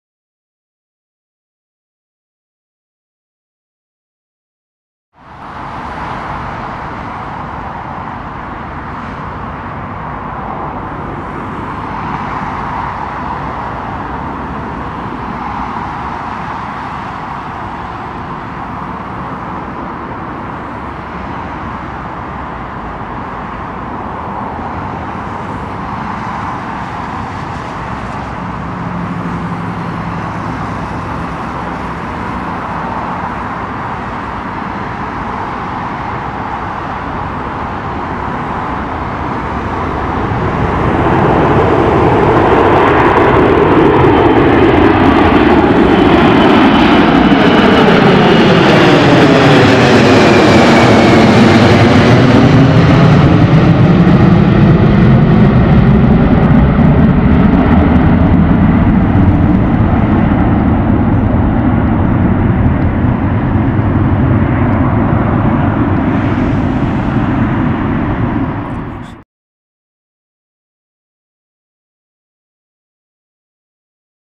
[LHR SPOTTING!] Korean 747-400 Departure! HD!
A Korean Boeing 747-400 Taking Off From London Heathrow Runway 09R.
This Was Probably One Of The Last Times That The Korean 747-400 Was Visiting LHR As It Is Usually A Mix Of A380 And 747-8, With The 747-400 Being Retired.
Filmed From Myrtle Avenue With A Nikon D3300.

This Video Is Property Of V1RotateAviation And May Not Be Used Without Written Permission From The Channel Owner.